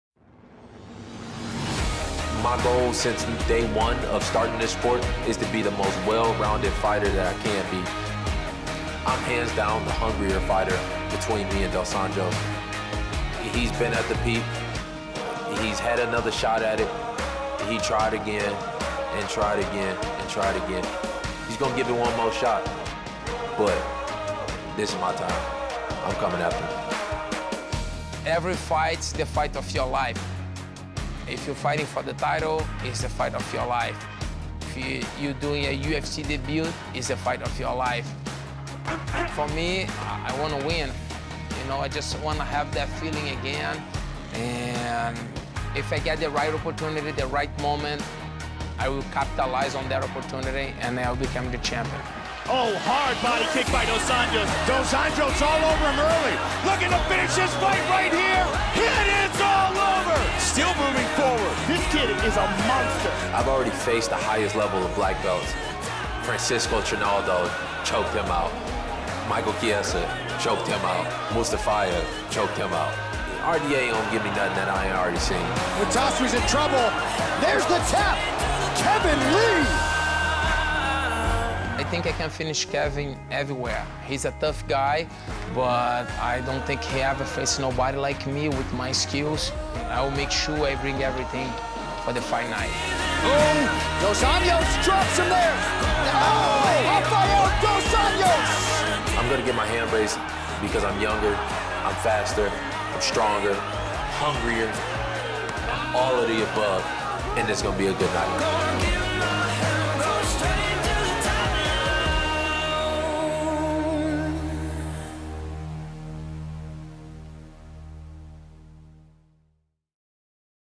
Fight Night Rochester: Rafael dos Anjos vs Kevin Lee - Straight to the Top
Rafael dos Anjos welcomes Kevin Lee to the welterweight division as they each make their best case for title contention this Saturday, May 18 on ESPN+.